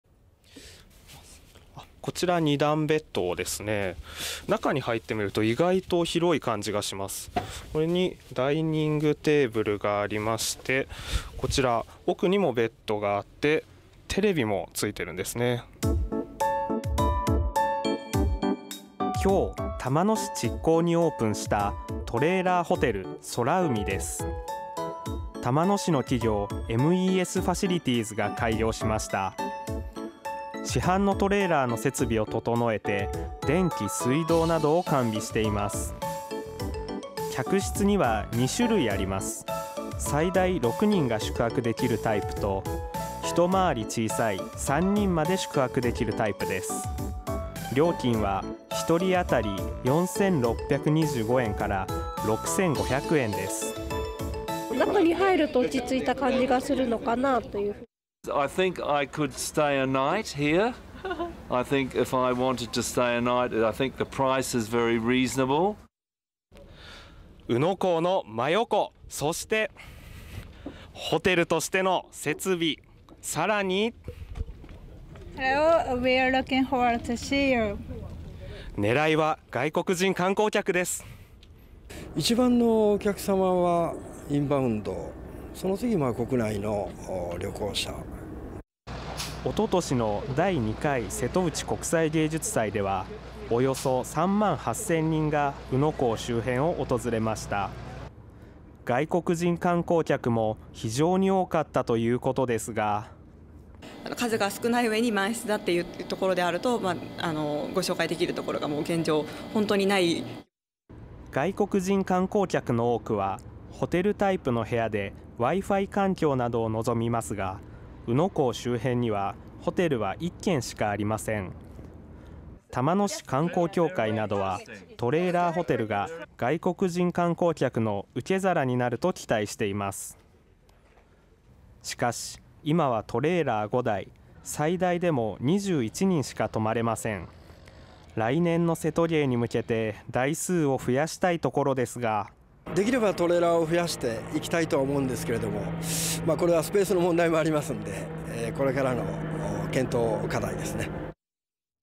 港近くの駐車場に「トレーラーホテル」オープン 狙いは外国人観光客！岡山
海のそばの駐車場がホテルになりました。岡山県玉野市にキャンピングカーのトレーラーを使ったホテルがオープンしました。